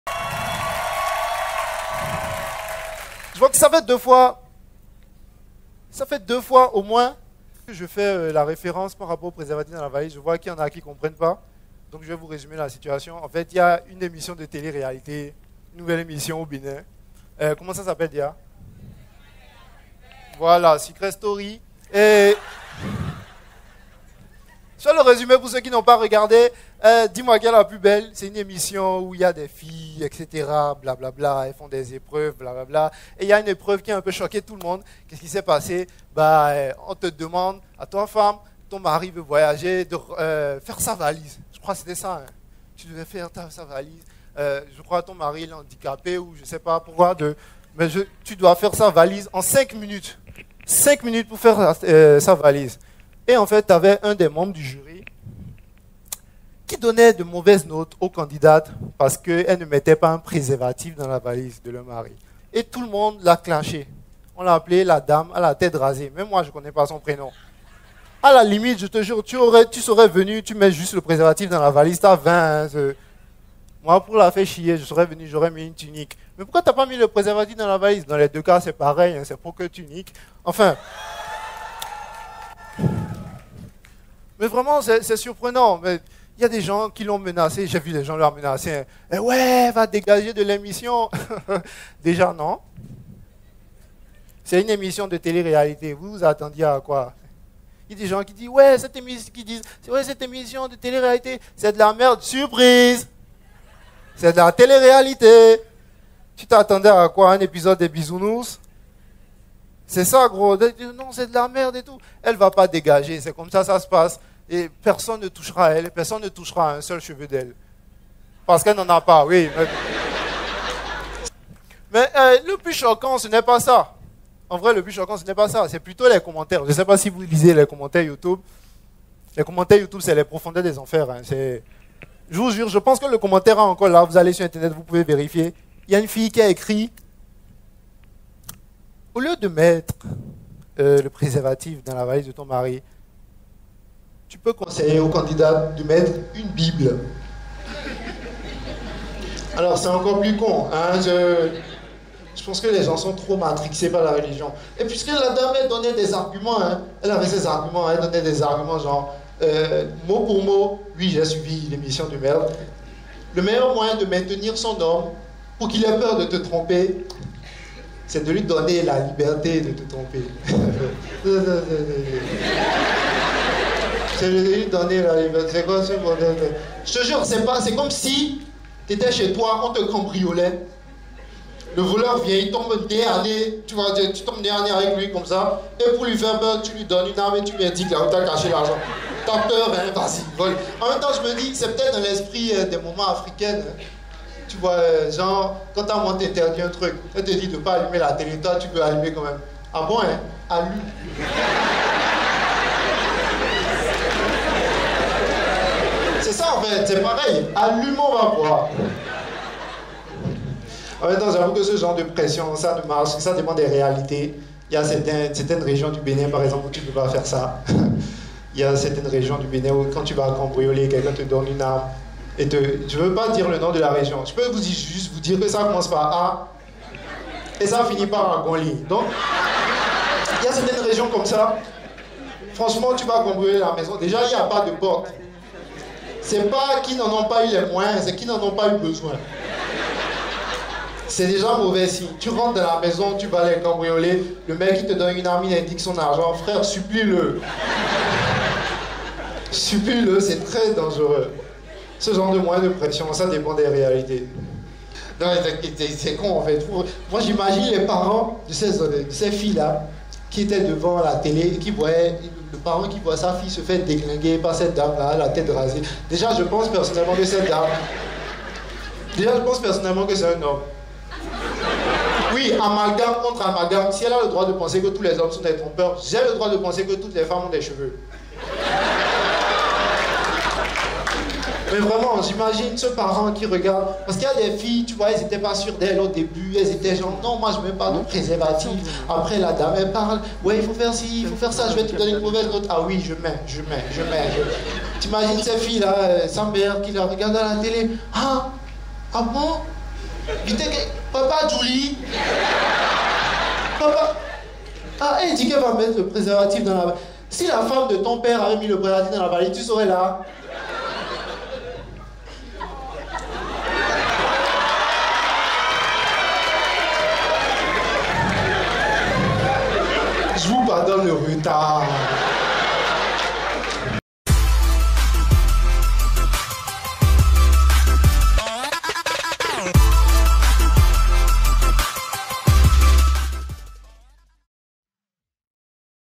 LA TÉLÉ RÉALITÉ AU BÉNIN. - Kenneth Yannick
Et vous? Auriez vous mis un préservatif dans la valise de voyage de votre partenaire ?

Extrait du spectacleDéfoncé

Retrouvez moi sur la scène de l’institut Français le 2 Mars à 20h30 .